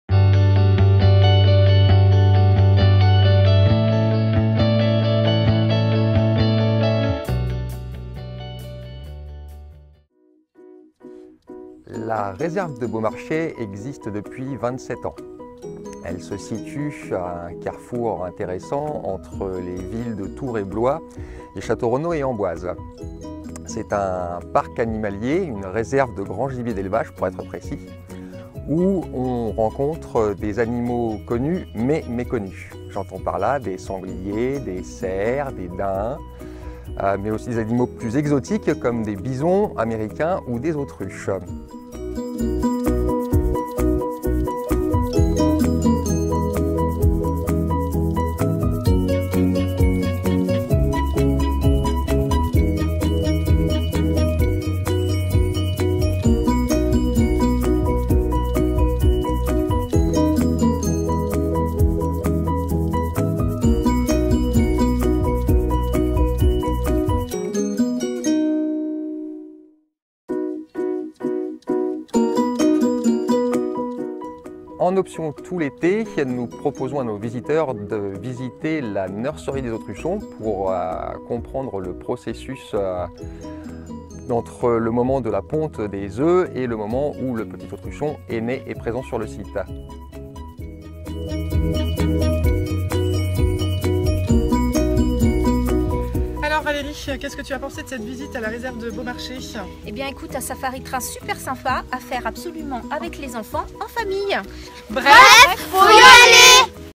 La réserve de Beaumarchais
A deux pas d'Amboise, embarquez à bord du Safari Train pour une étonnante traversée d'un parc animalier, à la rencontre des grands gibiers d'élevage !